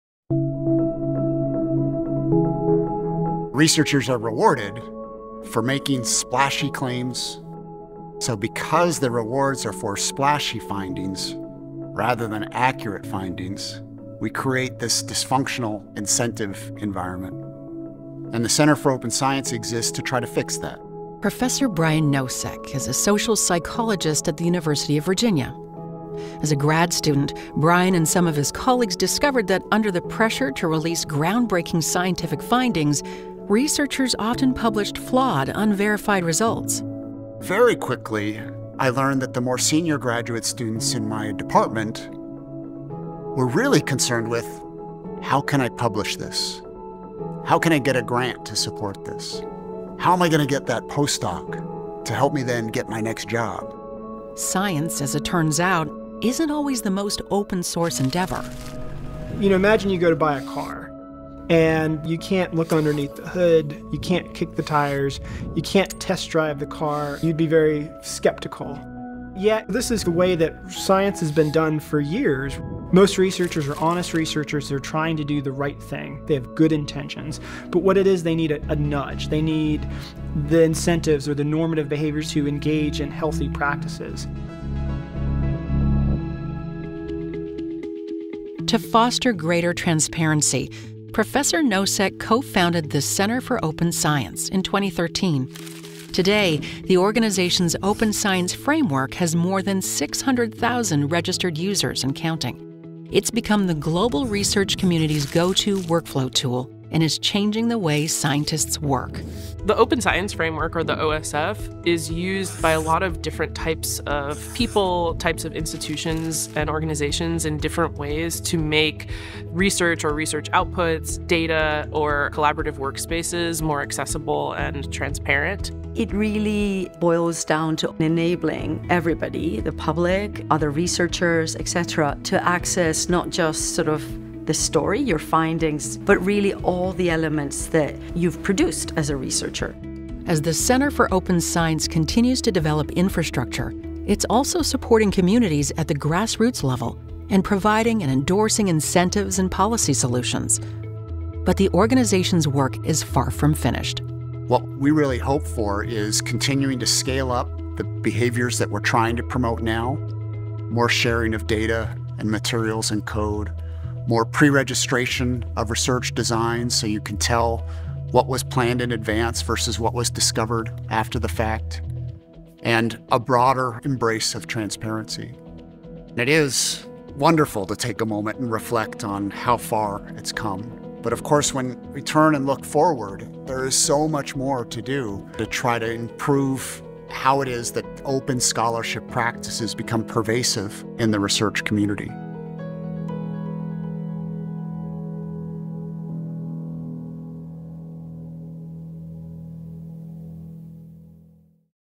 Advancing Research Transparency | Brian Nosek, Center for Open Science, Inc.
What if what we don’t know about scientific research could hurt us?
What if research practices were less competitive and more transparent?
What if the pace and quality of discovery could be improved?